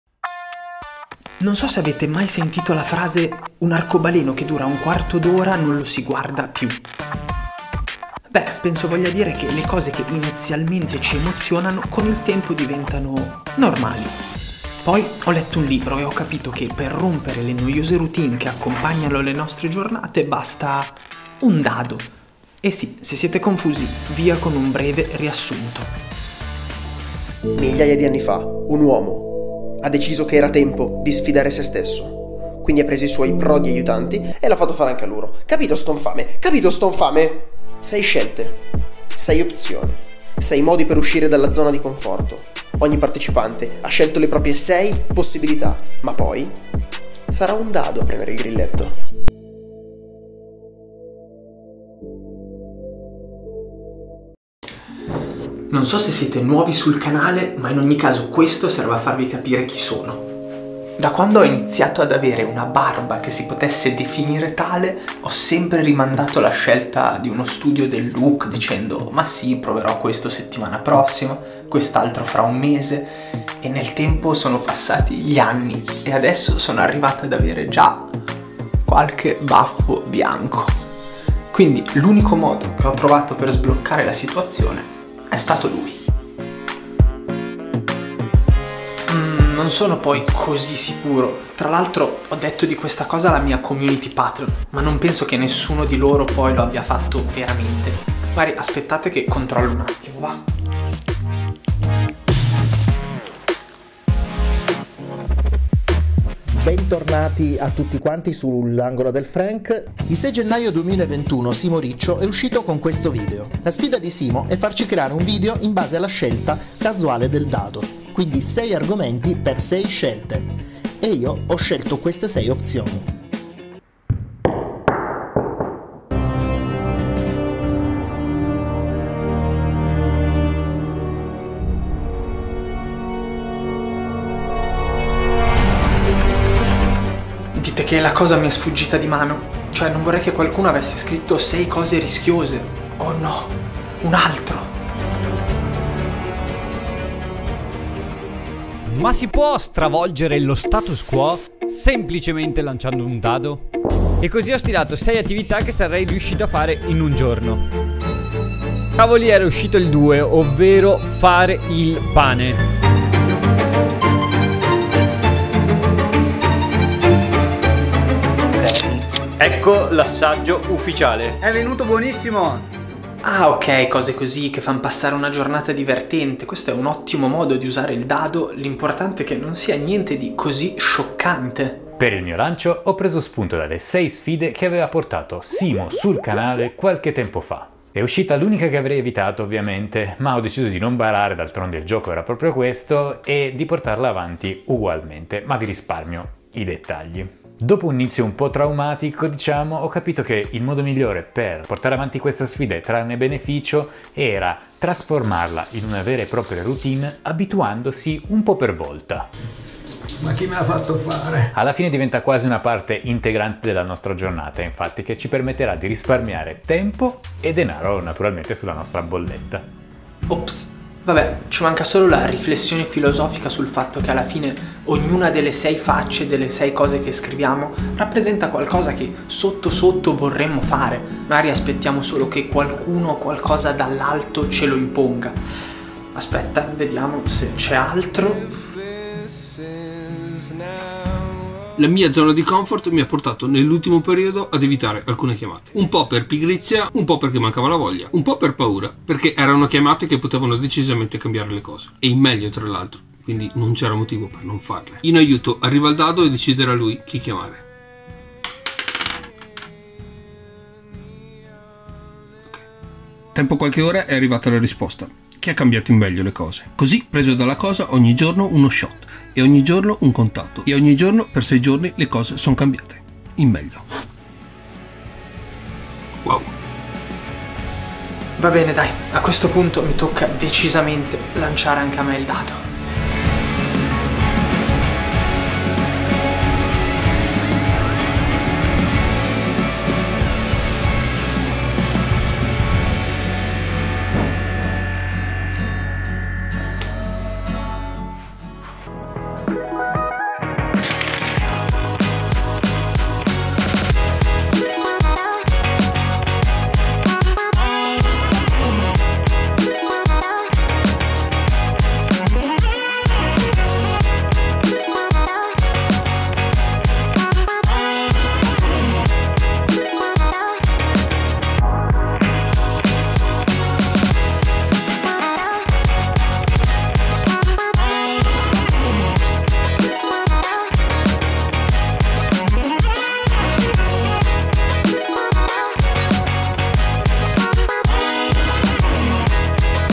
SMETTI DI FARE SEMPRE LE STESSE COSE 🎲 Cambia routine
Grazie ai partecipanti che hanno deciso di spezzare la loro routine (in ordine di apparizione):
@Marco Proserpio, @L'angolo del Frank, @sperimentatori , @DarioOrecchia , @Samuele Buson e @StreetLevelPhotography !

🖐 Vuoi partecipare ai miei prossimi video?

▬▬▬▬ ᴄᴏsᴀ ᴜsᴏ ᴘᴇʀ ʀᴇɢɪsᴛʀᴀʀᴇ ᴠɪᴅᴇᴏ
🎥 Videocamera Sony ➠
🎙 Microfono da PRO➠
🎤 Microfonino per la camera ➠
🎙 Hard disk ➠